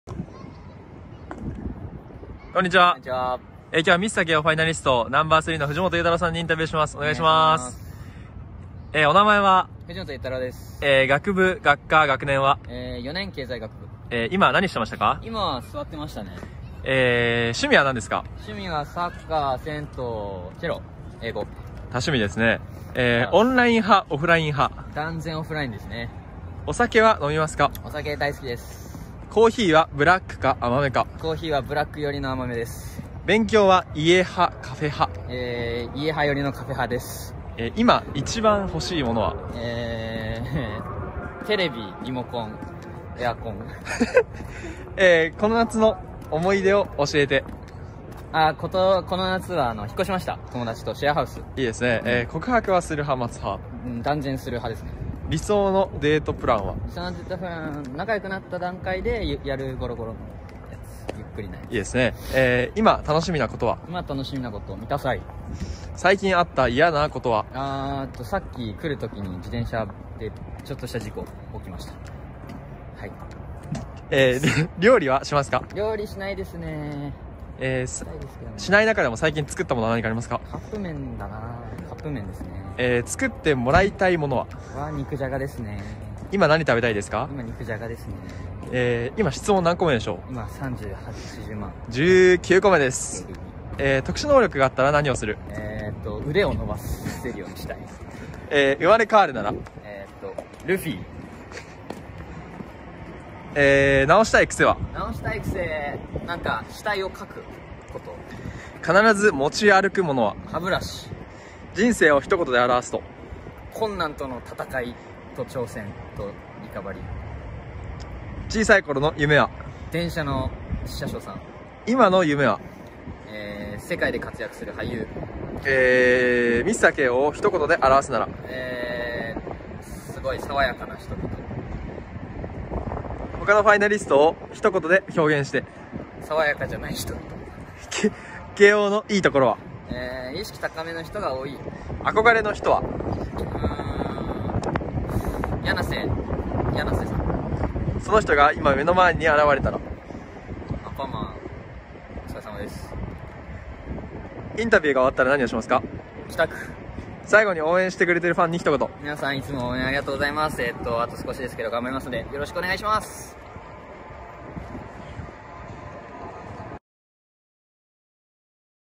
【ミスター慶應2020】33Q’s with Yutaro Fujimoto
6日連続でファイナリストのことがもっとよくわかる33の質問を載せていきます！
3日目はエントリーNo.3の藤本さんです！

mrkeio2020_no3